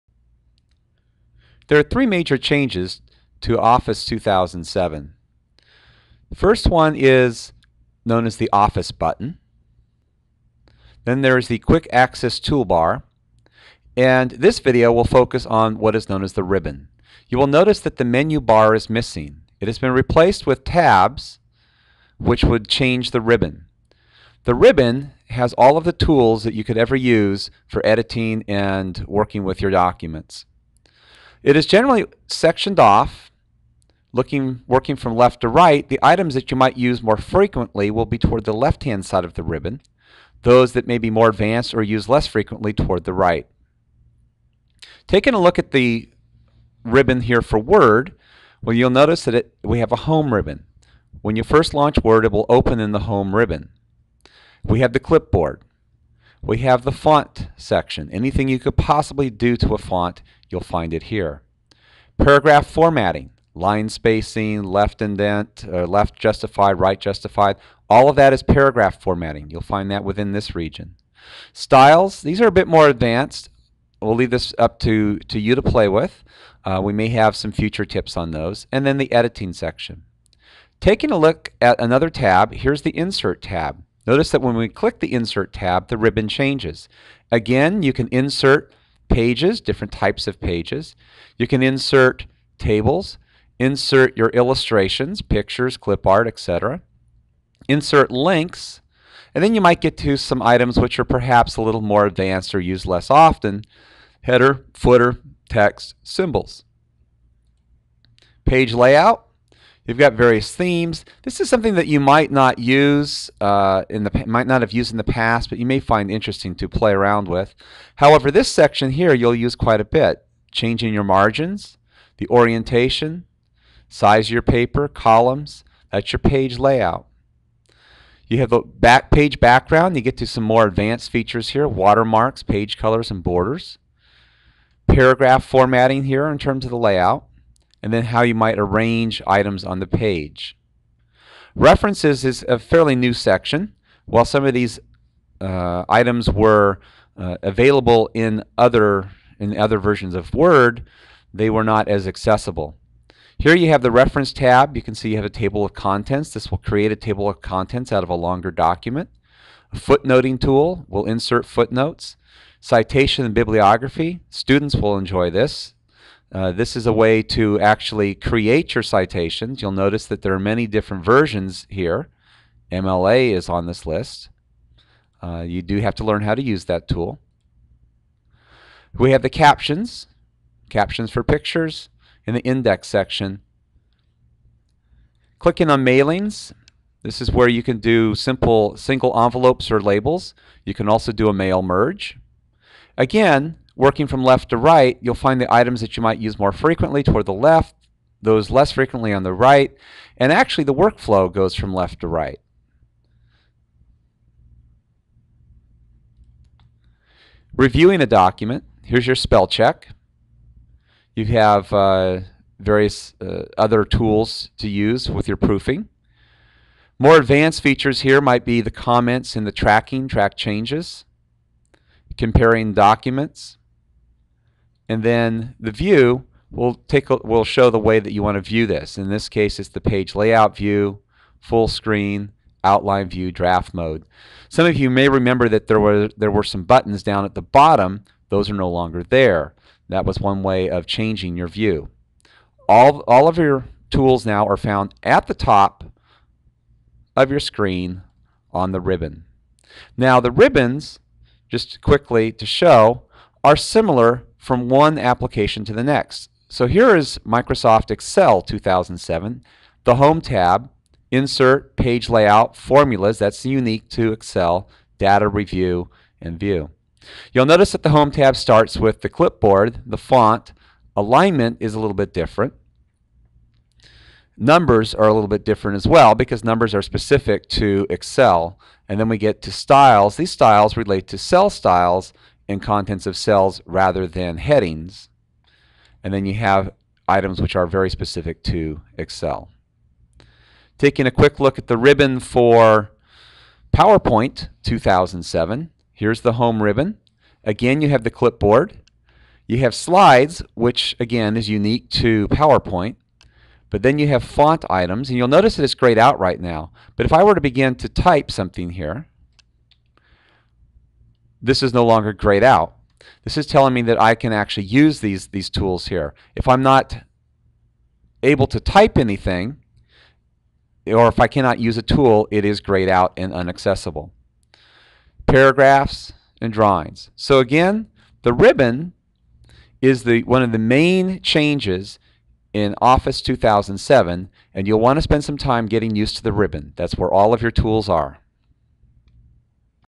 Welcome to MS Office Ribbon
This short video focuses on the Office Ribbon. The ribbon is probably the most significant difference between Office 2007 and prior versions of Office. You will notice that the menu bar is lacking in all Office 2007 programs with the exception of Outlook 2007. The ribbon is now your primary means of access to options and features in the Office Suite. This video is one in a series created as an orientation to the new MS Office 2007 work environment.